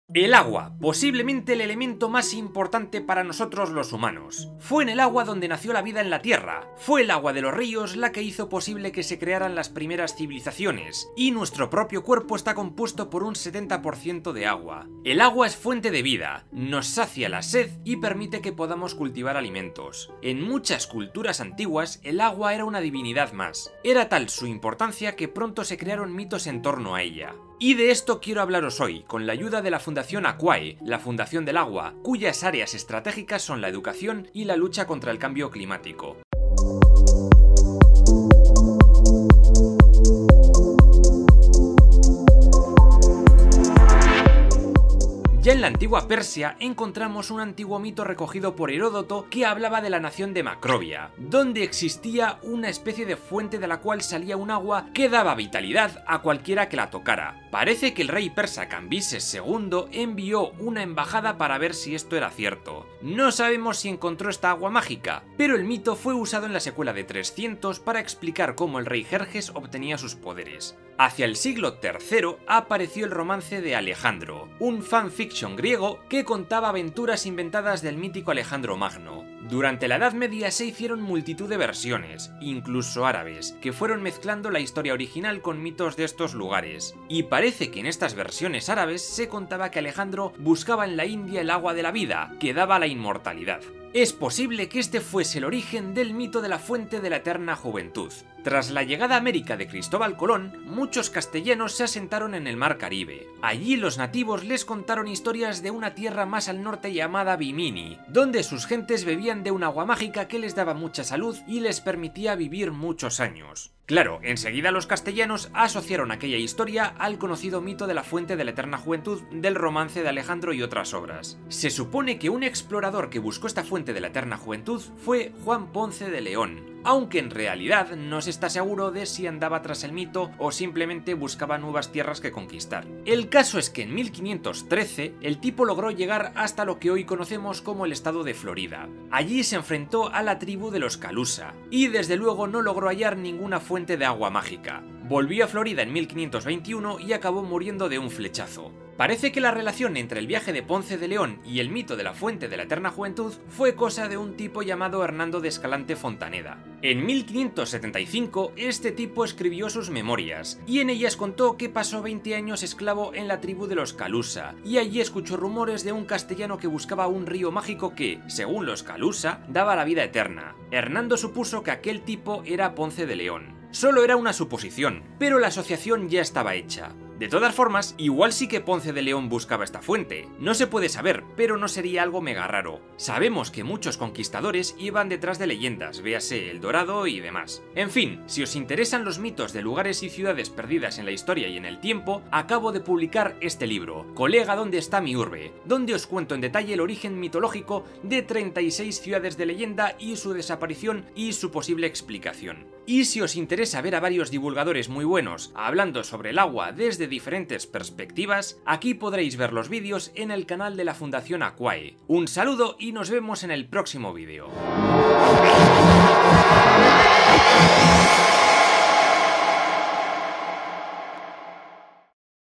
💧El mito de la fuente de la ETERNA JUVENTUD💧 | con ANDONI GARRIDO@PeroesoesotraHistoria | Campus
El agua es un recurso vital para los seres vivos. Andoni Garrido, escritor y divulgador en @PeroesoesotraHistoria, destaca su importancia a lo largo de nuestra historia y nos cuenta los diferentes mitos que pronto comenzaron a crearse sobre ella.

Andoni Garrido comienza este recorrido por los principales mitos sobre el agua en la Antigua Persia. En la nación Macrobio se creía en la existencia de una fuente de la cual emanaba un agua que daba vitalidad a cualquiera que la tocase. Fue el rey persa Cambises Segundo quien decidió enviar a una embajada para corroborar si aquello era cierto.

“No sabemos si el mito era cierto, pero fue utilizado en la famosa película de 300 para explicar como el rey Jerjes obtenía sus poderes” destaca el divulgador de Historia.

Fue en el siglo tercero cuando aparece el famoso romance de Alejandro que, como explica Garrido, “se trataba de un fan fiction griego que contaba aventuras inventadas del mítico Alejandro Magno”.

Durante la Edad Media fueron muchas las versiones que se hicieron sobre esta historia. Desde árabes hasta romanos fueron adaptando esta historia original a sus diferentes culturas y sociedades dando lugar multitud de versiones sobre la idea de que el agua era la fuente de la vida eterna.

Precisamente, una de estas versiones realizadas por los árabes, aseguraba que Alejandro Magno viajaba con sus siervos a la India en busca de una fuente de agua que le otorgaría la inmortalidad.